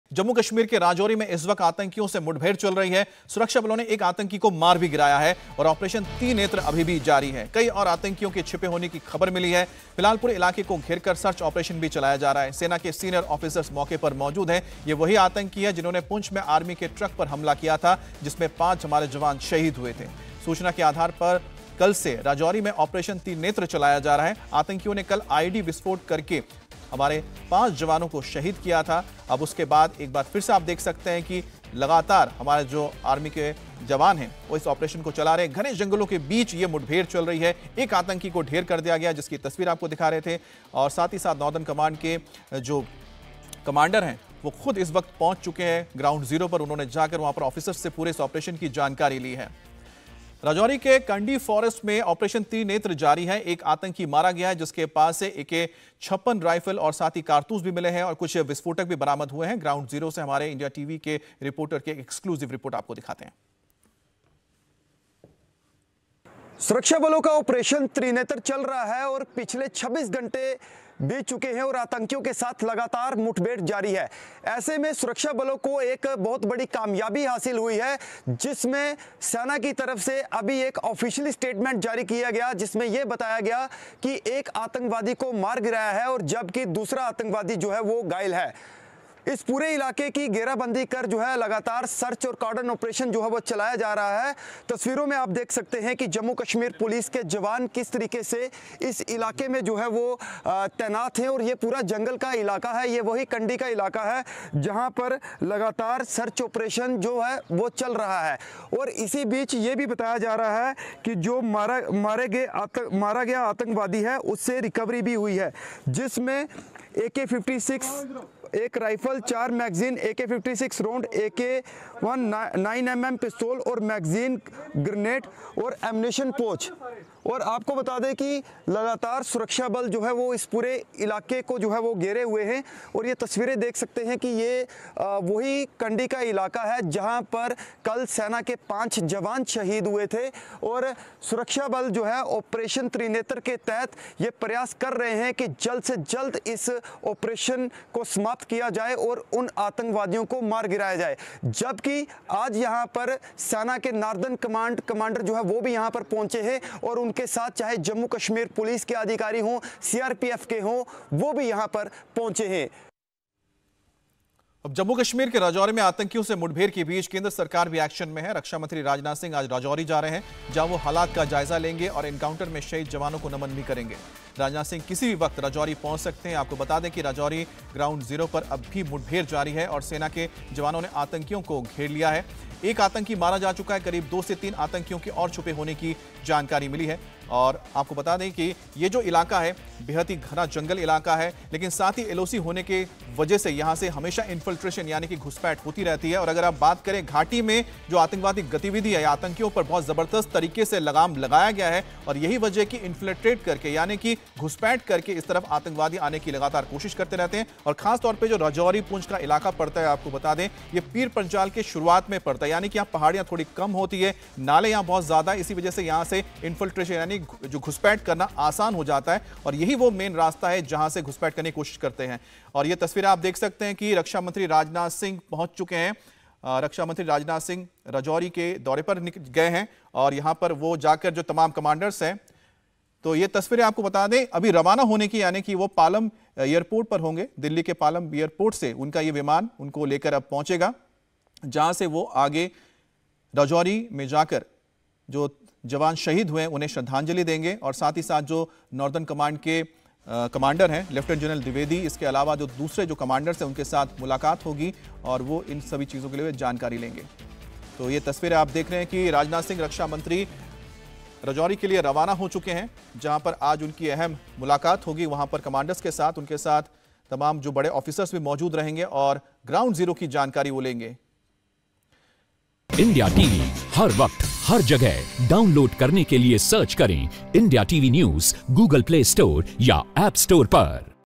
Rajouri Encounter: राजौरी में आतंकियों से मुठभेड़ जारी.. हालात का जायजा लेंगे Rajnath Singh | News
Rajouri Encounter: जम्मू कश्मीर के राजौरी में इस वक्त आतंकियों से मुठभेड़ चल रही है. सुरक्षाबलों ने एक आतंकी को मार गिराया है और ऑपरेशन त्रिनेत्र जारी है